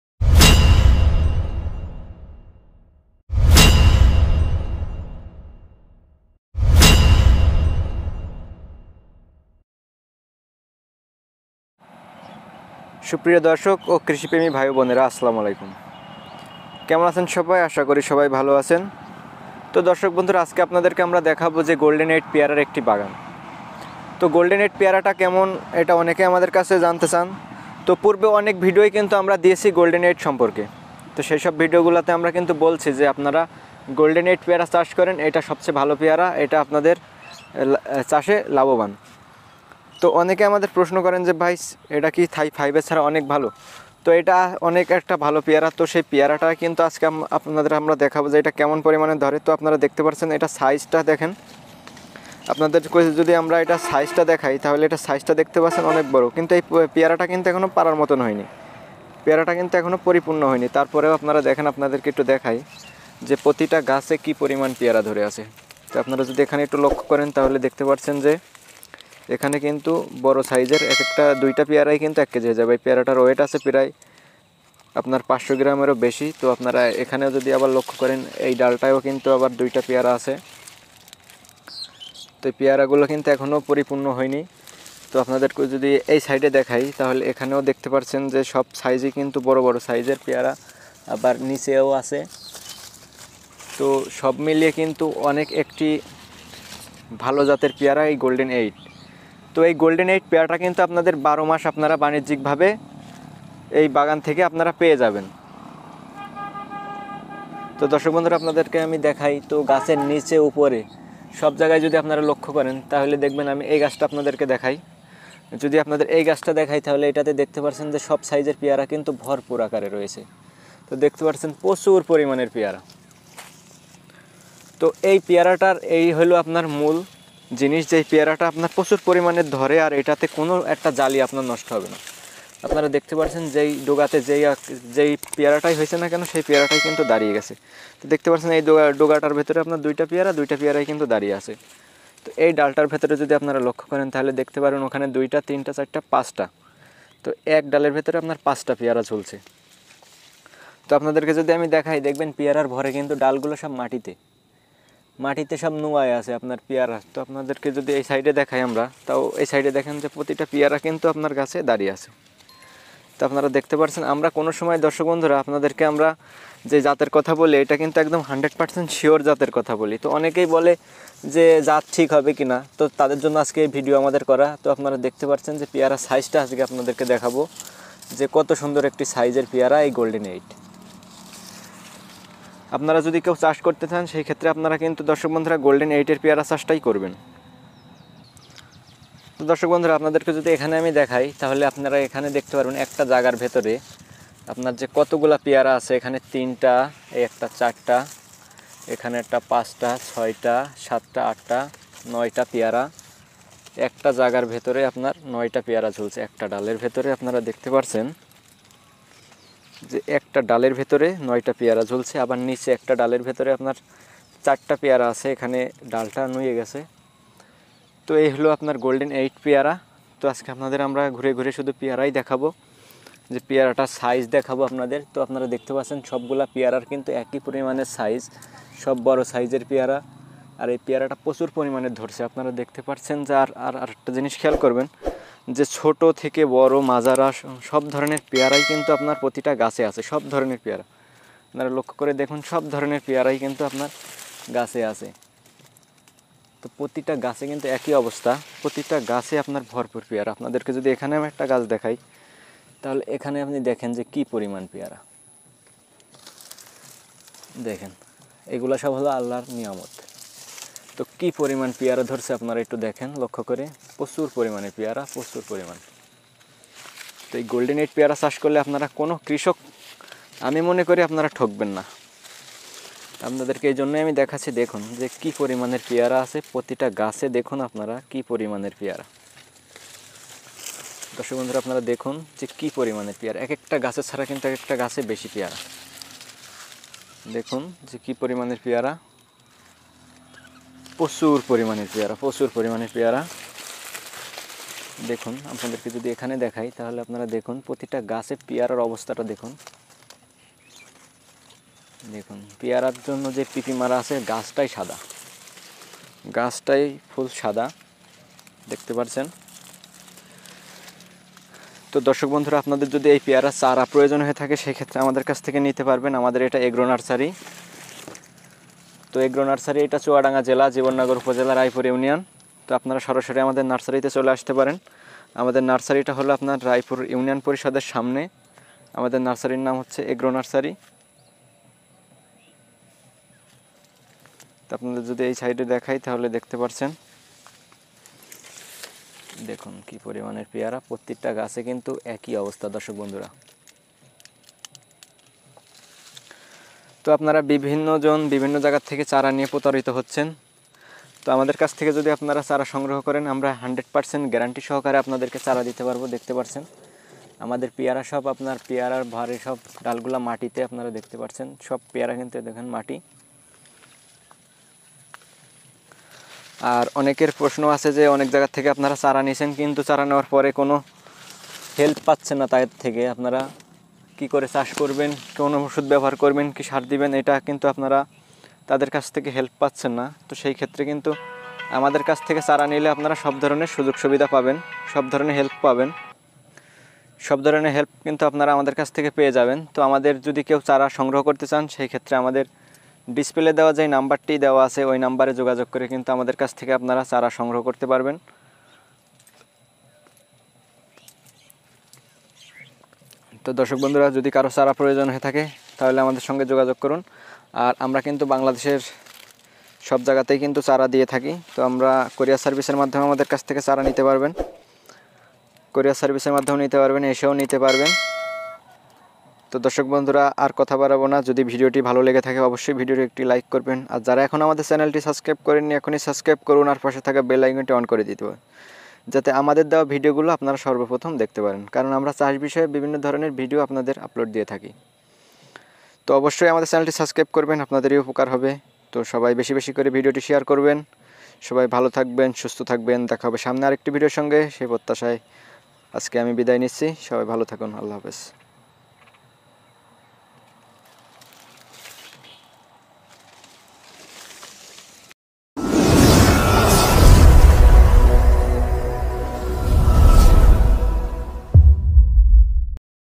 পেয়ারার ভারে গাছের ডাল মাটিতে গোল্ডেন এইট পেয়ারা
গোল্ডেন এইট পেয়ারার চারার জন্য যোগাযোগ এগ্রো নার্সারি চুয়াডাঙ্গা জেলা জীবননগর উপজেলার রায়পুর ০১৯৭৪৪৮২৮০৬
ইমো,০১৯৭৪৪৮২৮০৬